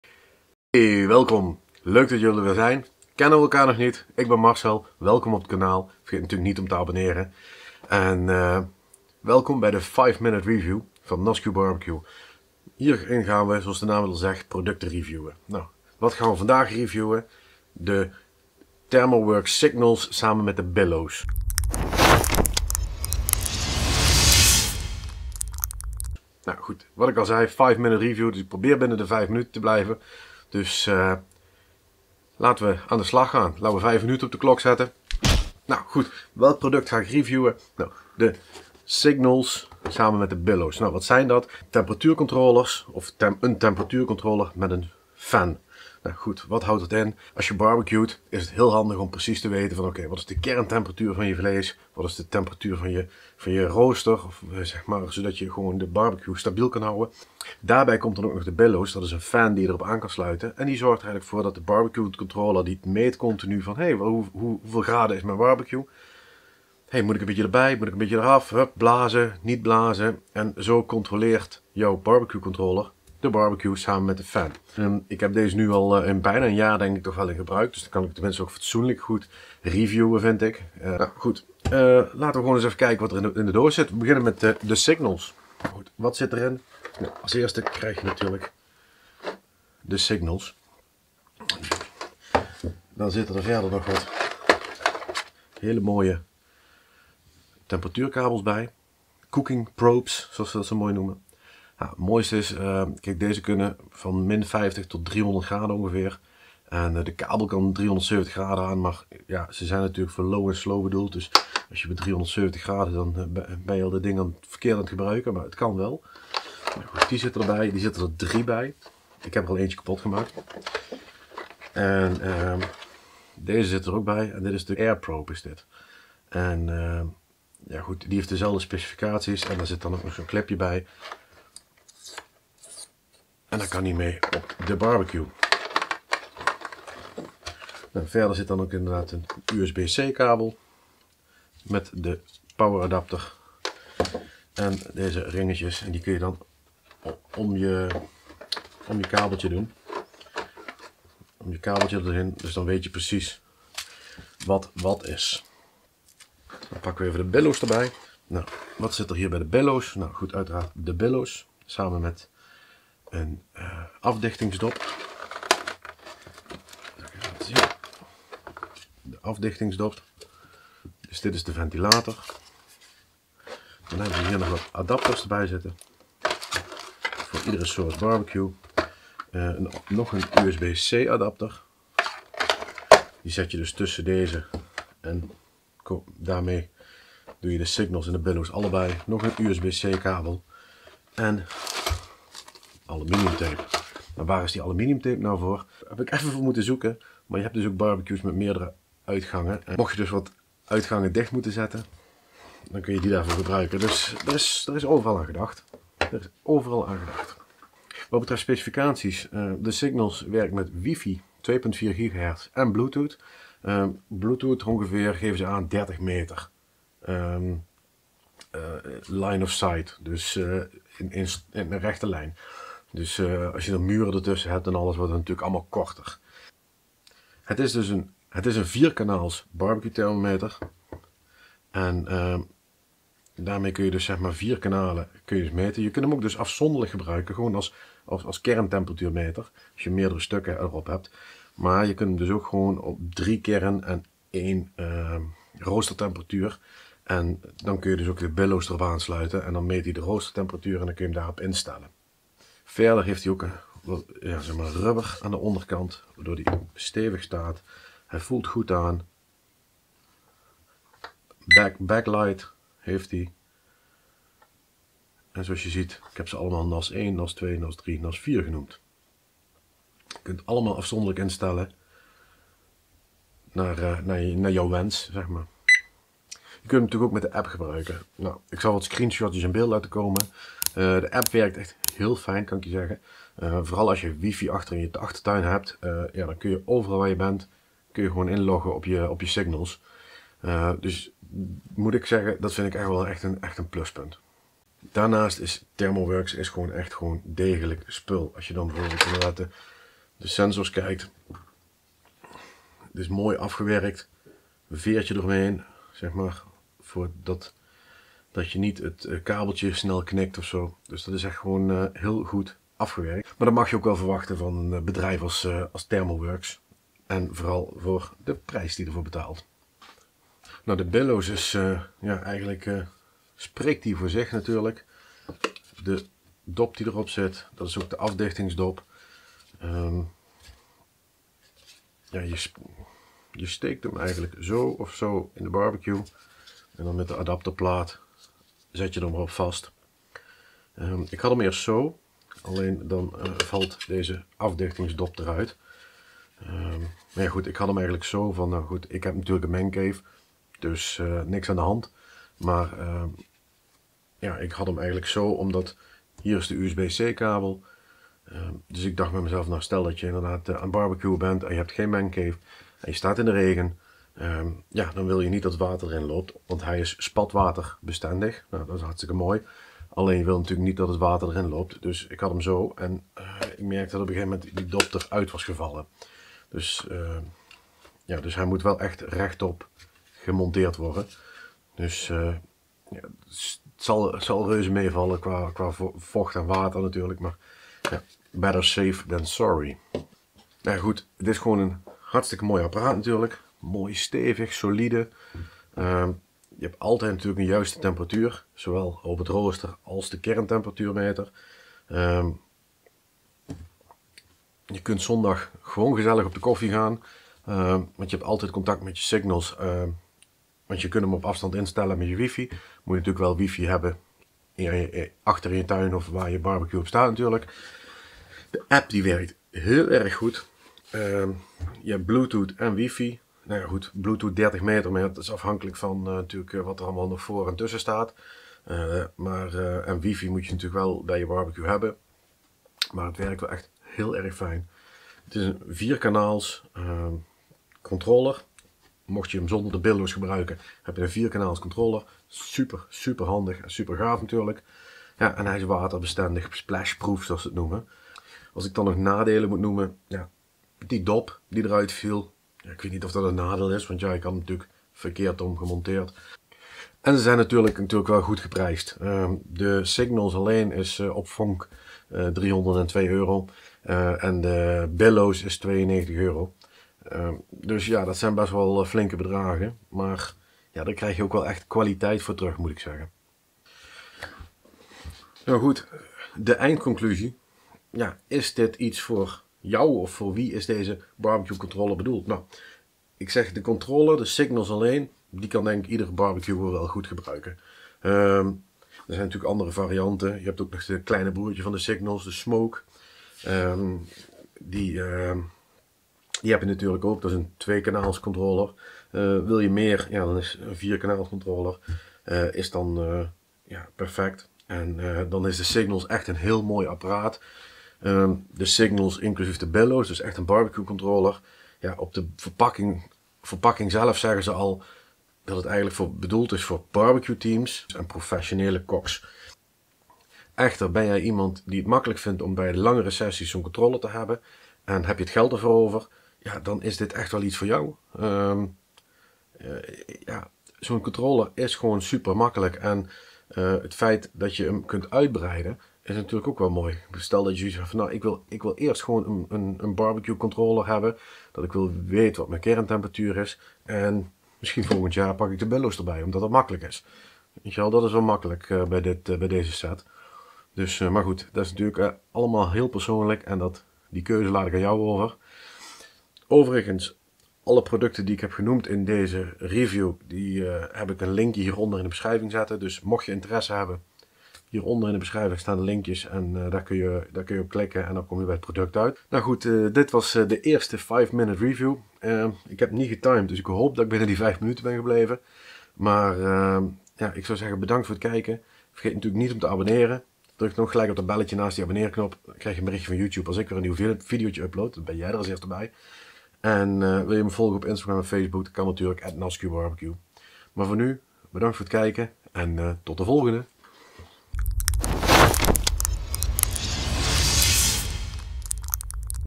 Thermoworks Signals & Billows - 5 minute review - NasQ BBQ
Welkom bij de 5-minute review op het NasQ BBQ kanaal! In deze videoreeks probeer ik binnen 5 minuten een review te doen van apparaten, gadgets, must-haves en nog veel meer op het gebied van BBQ!

We trappen af met de Thermoworks Signals & Billows.

Links van de producten:
Thermoworks Signals:

Wist je dat NasQ BBQ ook te vinden in op Social Media?

Benieuwd waar je NOSKOS saus en rubs kunt kopen?

- Youtube Mediabibliotheek